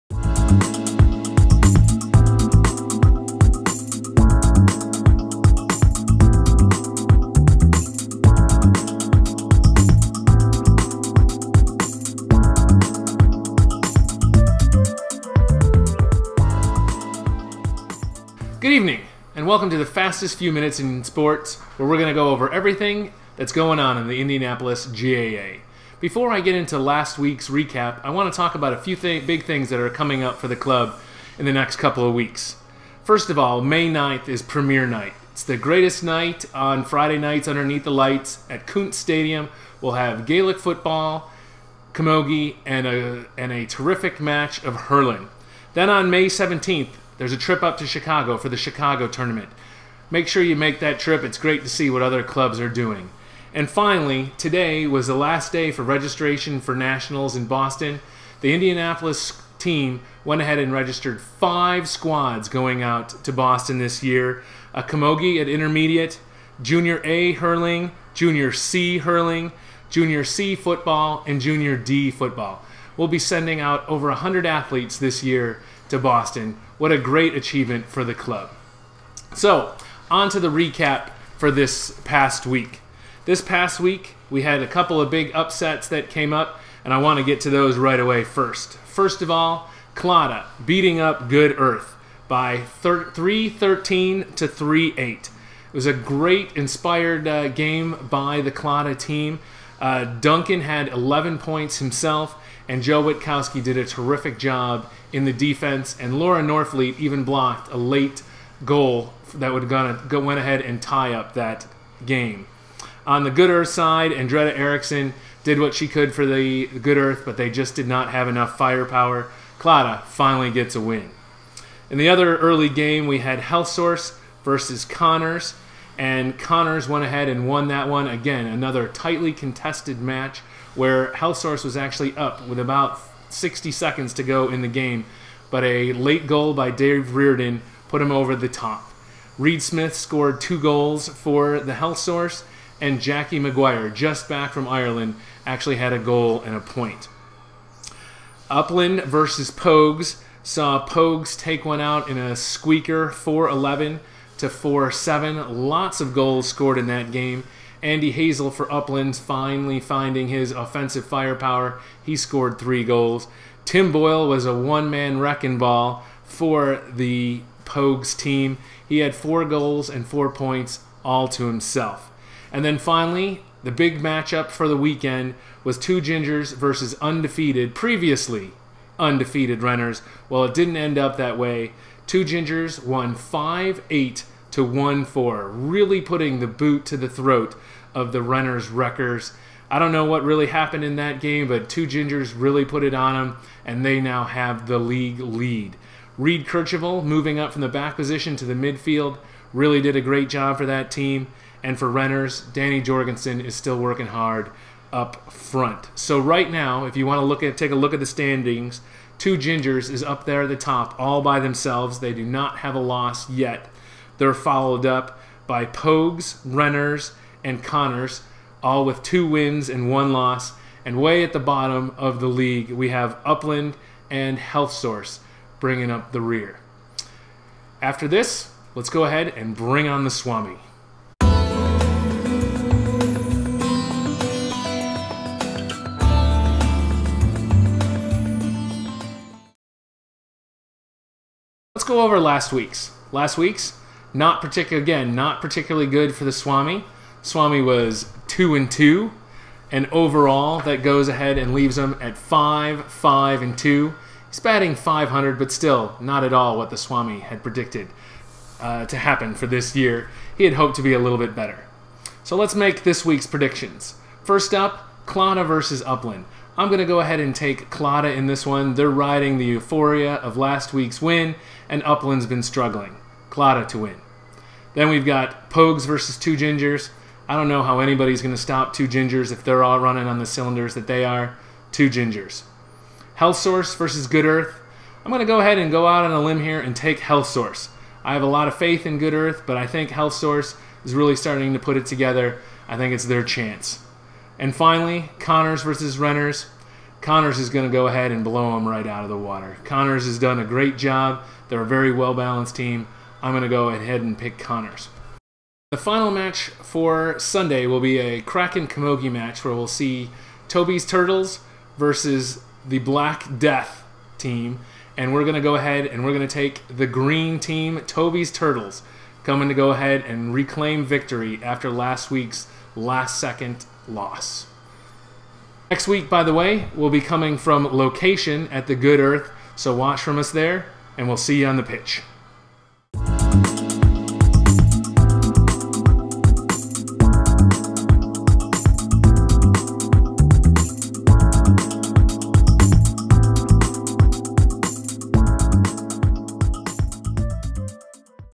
Ep. 4 Indianapolis GAA Recap and Swami Sez
Some Upcoming Club News, a Recap of the third round of Hurling games within the Indianapolis GAA Summer League 2014, and  Predictions at the end by the Swami!